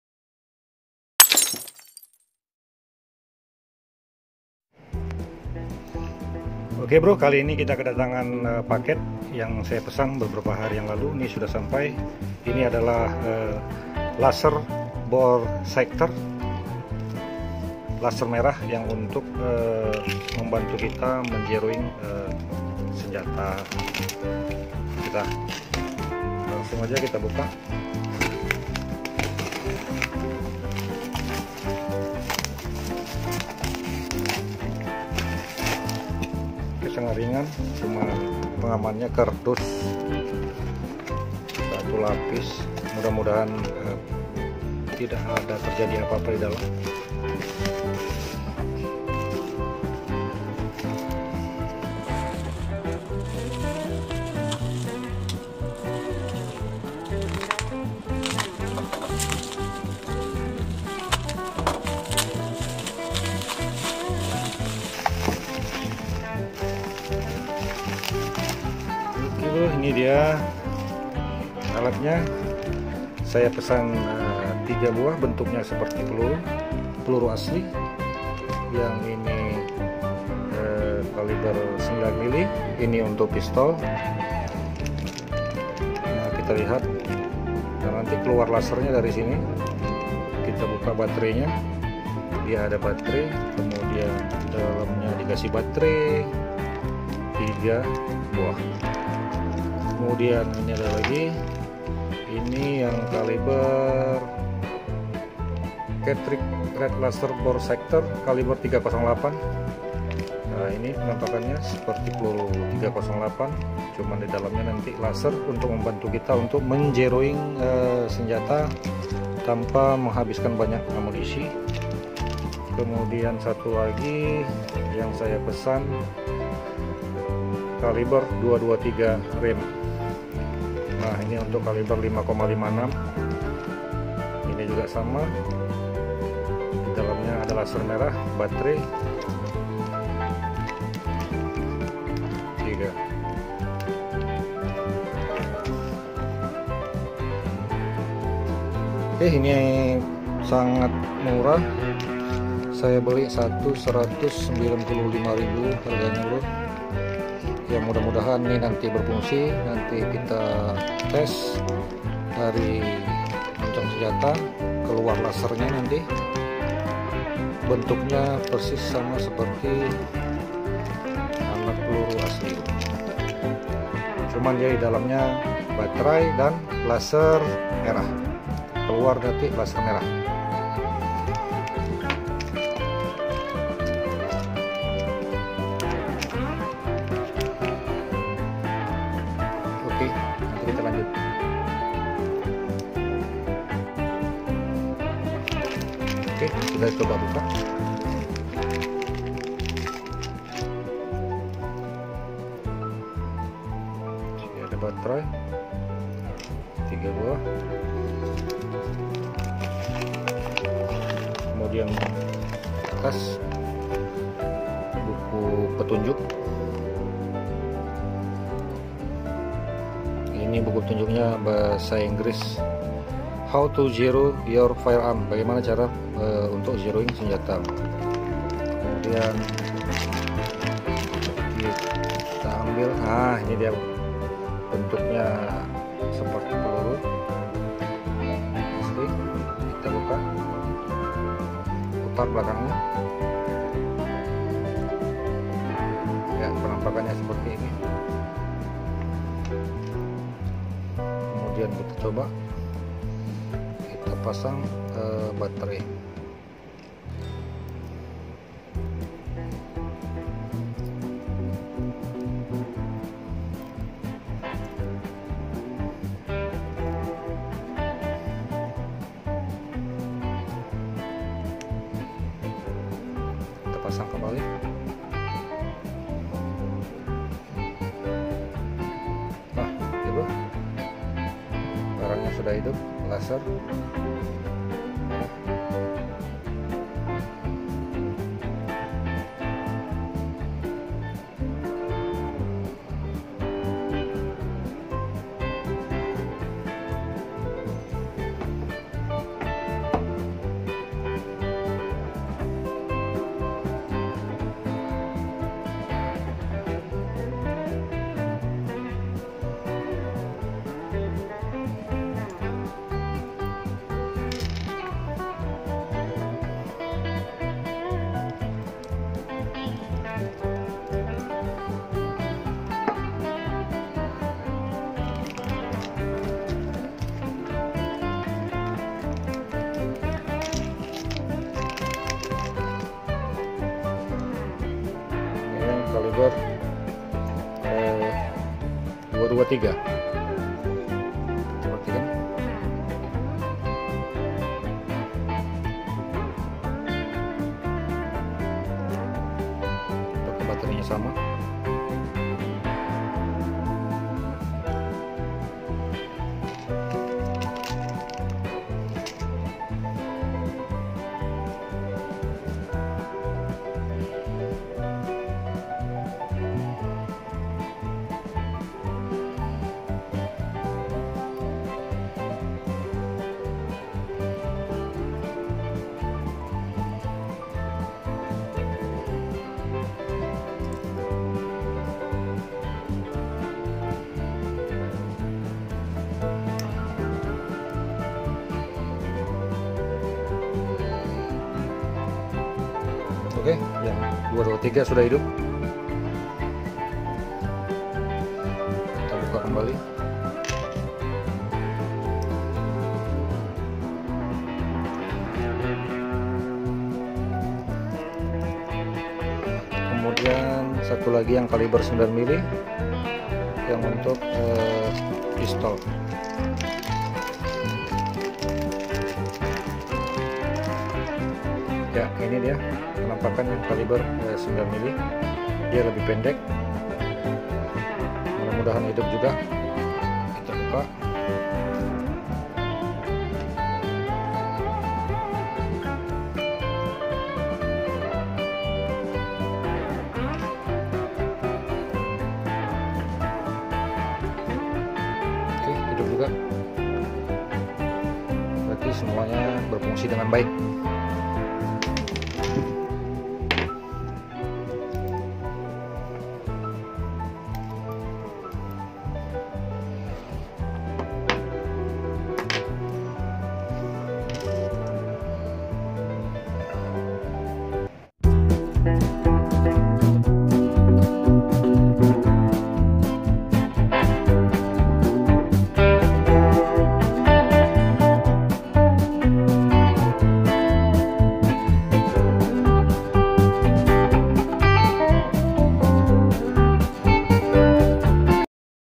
Unboxing mainan baru [Solusi mudah & cepat zeroing
Berikut ini hanya video unboxing barang mainan baru, solusi cepat dan mudah untuk menzeroing tele senjata.

Video ini hanya hiburan semata dan jangan dianggap sebagai instruksi ataupun saran .. TRIMS 🙏🙏🙏🙏